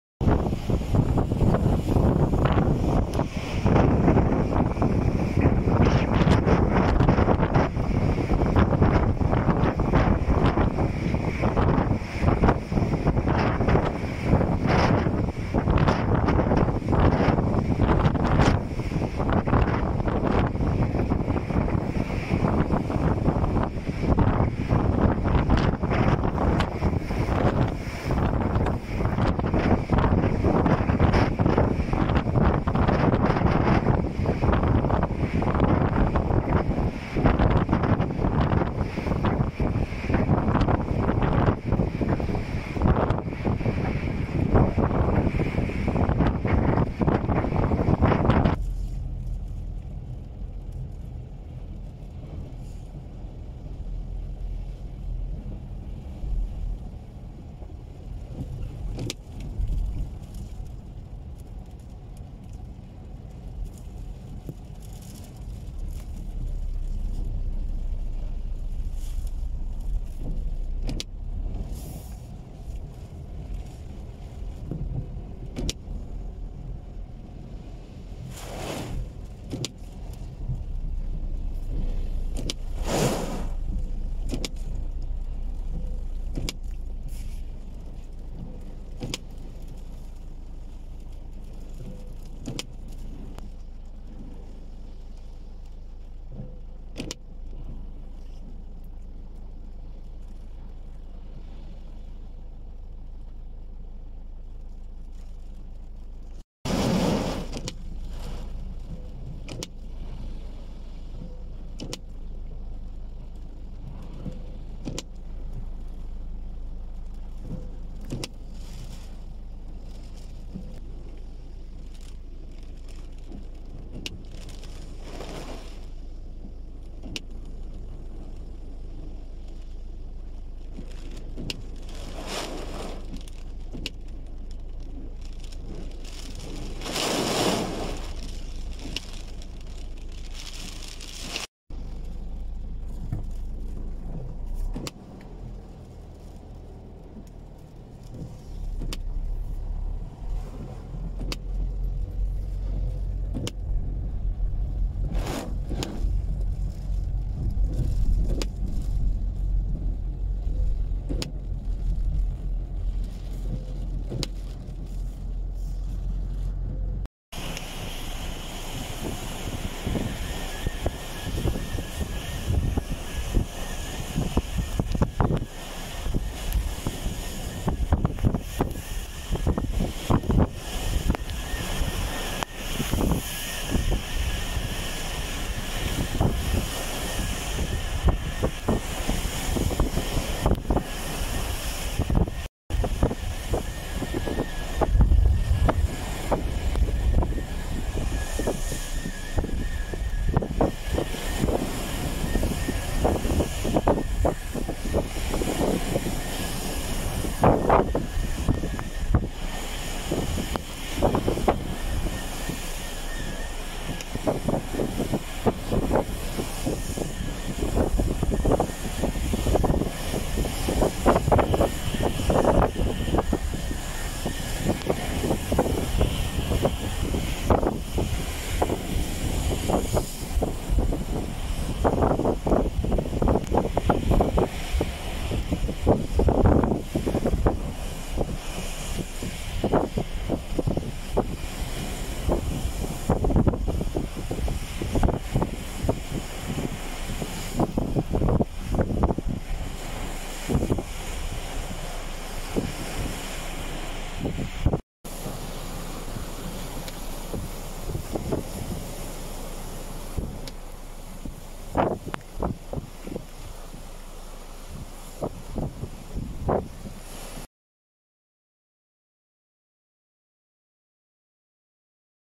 Orkansko jugo zahvatilo je tijekom noći Kaštela
Tijekom noći dalmaciju je zahvatilo orkansko jugo koje je stvorilo probleme u pomorskom prometu te je potopilo rivu u Kaštel Novom.